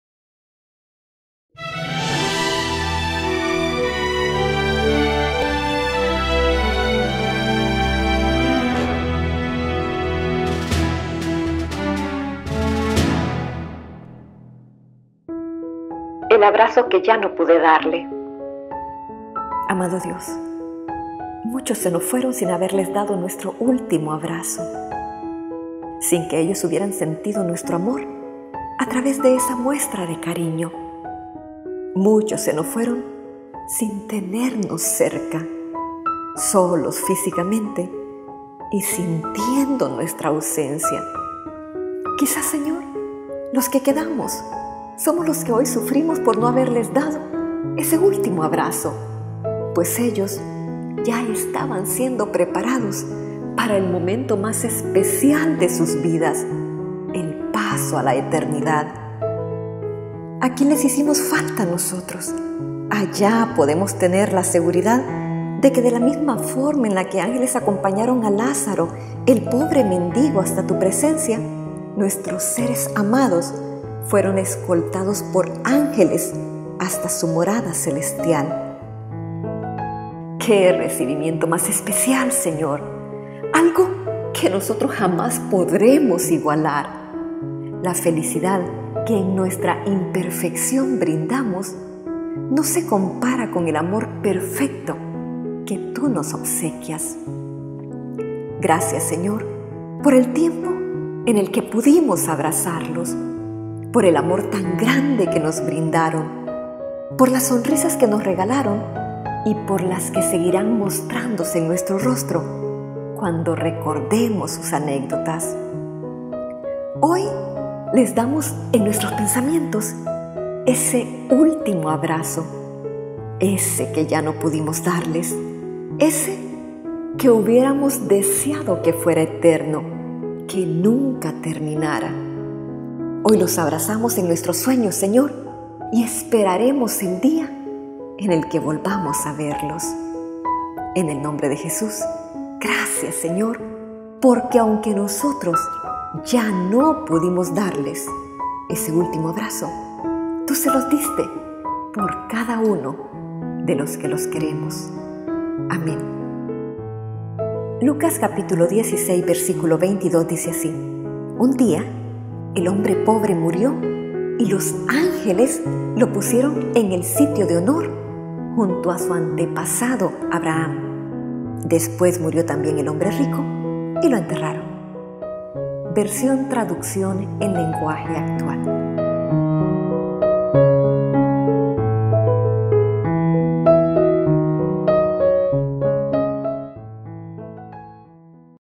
ORACIONES PARA REFLEXIONAR . "EL ABRAZO QUE YA NO PUDE DARLE"
*EL ABRAZO QUE YA NO PUDE DARLE*
Amado Dios, muchos se nos fueron sin haberles dado nuestro último abrazo; sin que ellos hubieran sentido nuestro amor, a través de esa muestra de cariño; muchos se fueron sin tenernos cerca, sólos físicamente y sintiendo nuestra ausencia.
Quizás Señor, los que quedamos, somos los que hoy sufrimos por no haberles dado, ese último abrazo; pues ellos, ya estaban siendo preparados para el momento más especial de sus vidas, el paso a la eternidad.
Aquí les hicimos falta nosotros, allá podemos tener la seguridad de que de la misma forma en la que ángeles acompañaron a Lazaro, el pobre mendigo hasta Tu presencia, nuestros seres amados, fueron escoltados por ángeles hasta su morada celestial; que recibimiento más especial Señor, algo que nosotros jamás podremos igualar. La felicidad que en nuestra imperfección brindamos, no se compara con el amor perfecto que Tú nos obsequias. Gracias Señor por el tiempo en el que pudimos abrazarlos, por el amor tan grande que nos brindaron, por las sonrisas que nos regalaron y por las que seguirán mostrándose en nuestro rostro, cuando recordemos sus anécdotas.
Hoy les damos en nuestros pensamientos, ese último abrazo, ese que ya no pudimos darles, ese que hubiéramos deseado que fuera eterno, que nunca terminara. Hoy los abrazamos en nuestros sueños Señor y esperaremos el día en el que volvamos a verlos.
En el nombre de Jesús, gracias Señor, porque aunque nosotros ya no pudimos darles… ese último abrazo… Tú se los diste, por cada uno, de los que los queremos.
Amén.
Lucas 16:22 (TLA) Un día, el hombre pobre murió y los ángeles lo pusieron en el sitio de honor, junto a su antepasado Abraham. Después murió también el hombre rico, y lo enterraron.